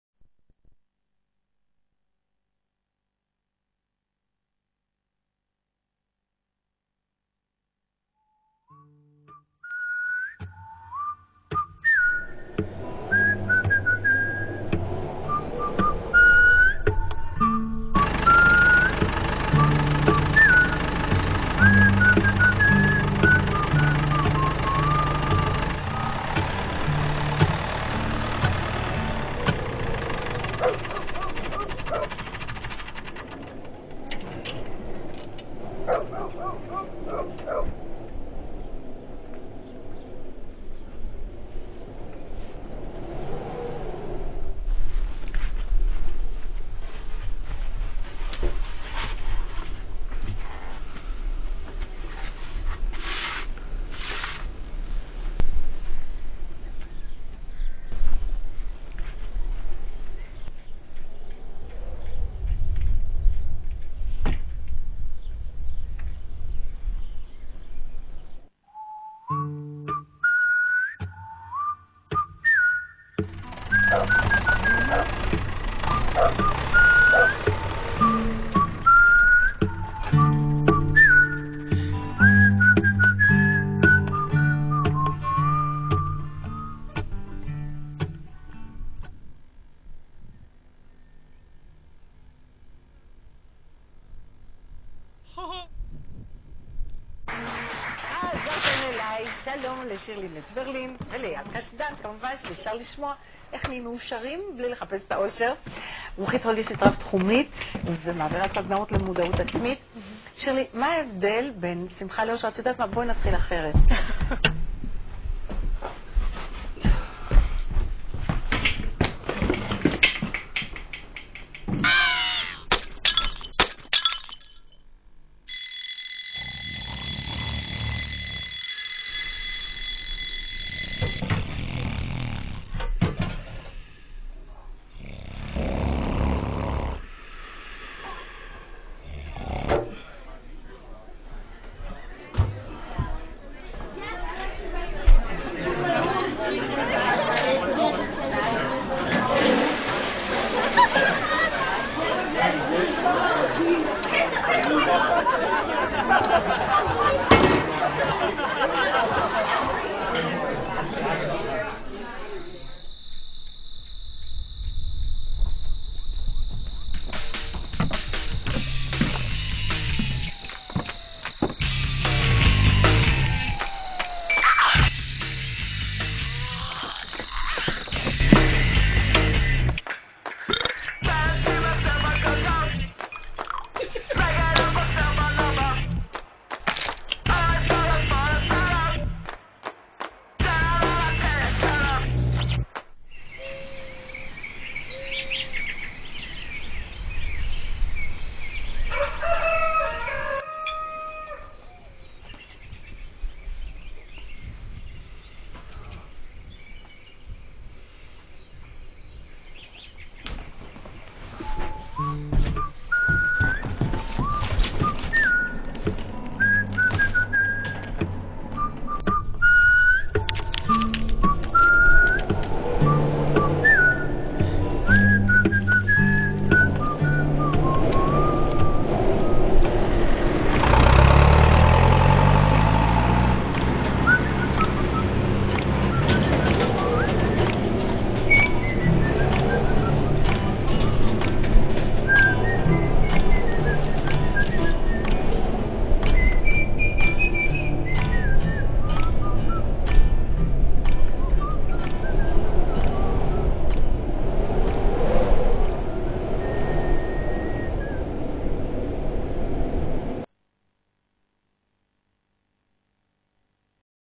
רעש מאומה
תסריט חפץ קצרצר ללא דיאלוג |
הופק בבית ספר סם-שפיגל |
תסריט - אלון רותם |